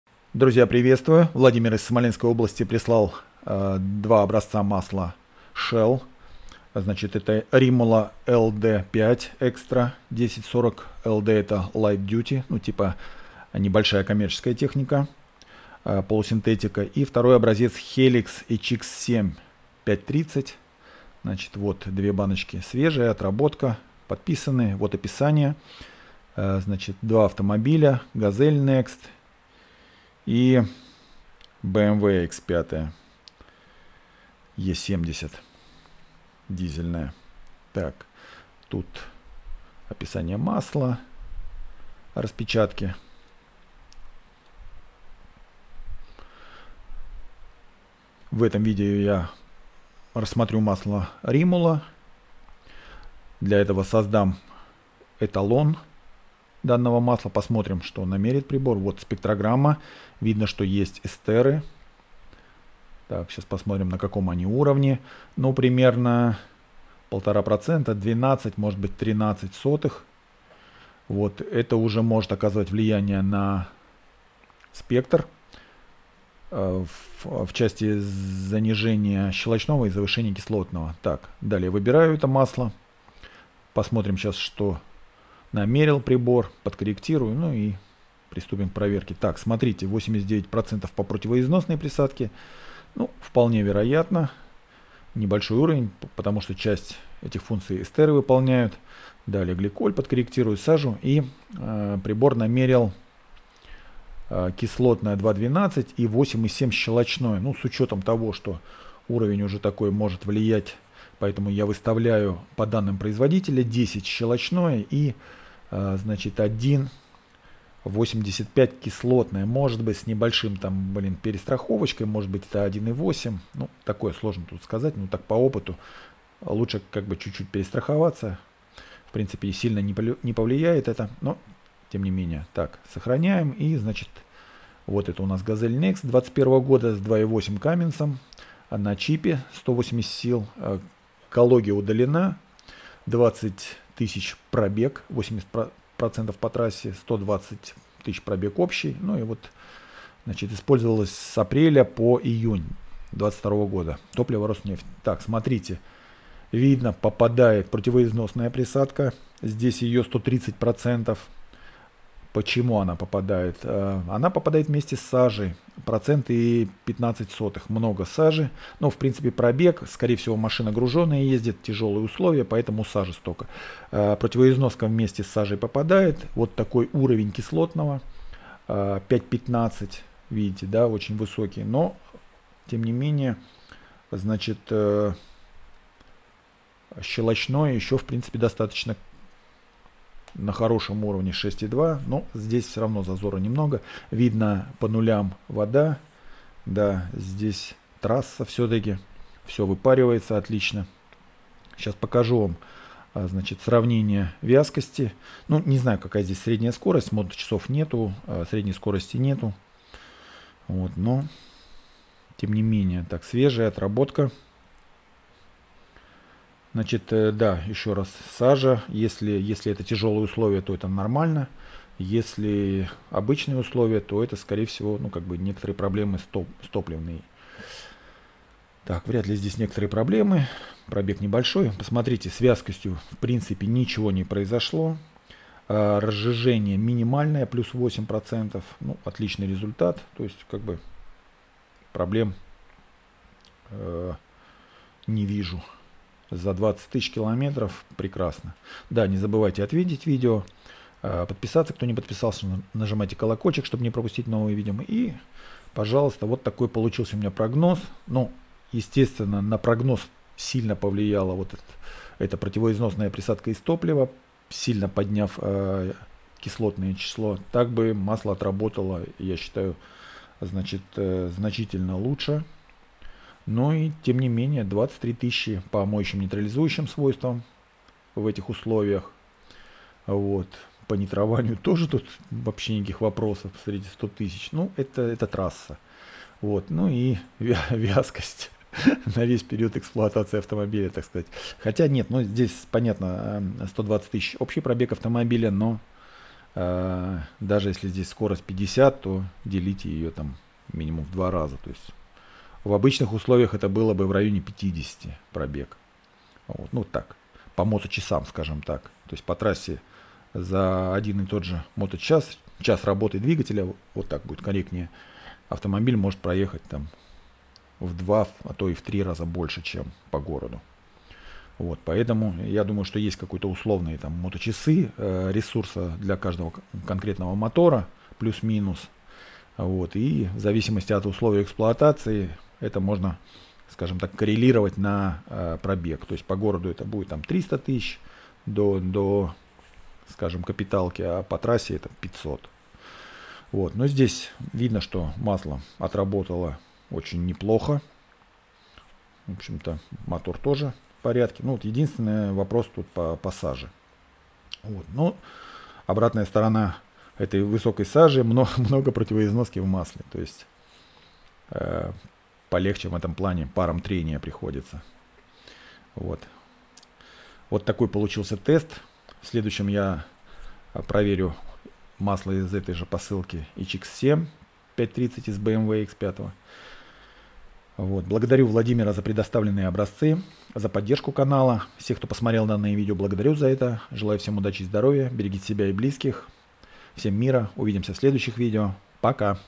Shell Rimula LD5 Extra 10w40 (отработка из Газели, 20 000 км., турбодизель Cummins).
Владимир из Смоленской области, прислал образцы масла Shell Rimula LD5 Extra 10w40

свежее и отработку с пробегом 20 000 км. (80% по трассе).
Масло использовались в  автомобиле Gazelle Next, 2021 г.в.
с двигателем 2,8  (Cummins), 180 сил.
прошивка stage - 1, экология удалена.
использовалось с апреля 2022 по июнь 2022 гг.

без доливки

топливо Роснефть (ДТ)

Благодарю Владимира за предоставленные образцы и помощь каналу!